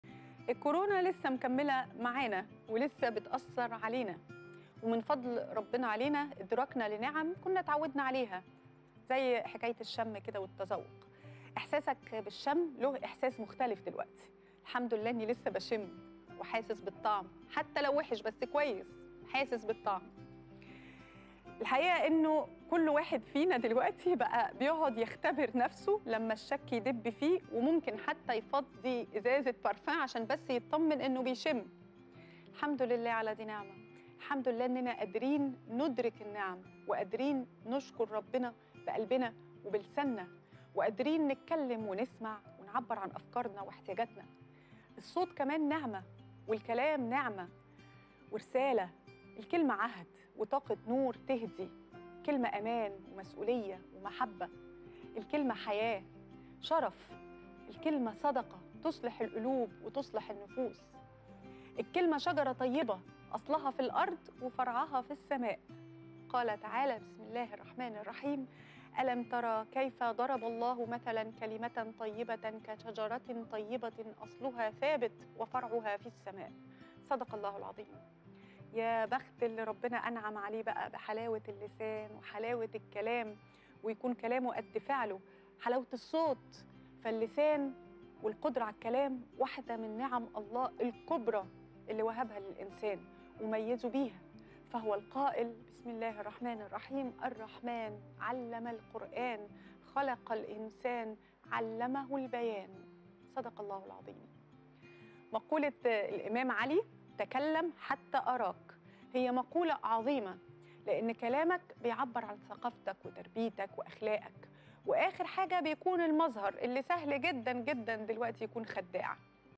إيمان رياض: يابخت إللي ربنا أنعم عليه بحلاوة اللسان والكلام ويكون كلامه زي فعله
تابع جميع الحلقات على Shahid VIP واشترك الآن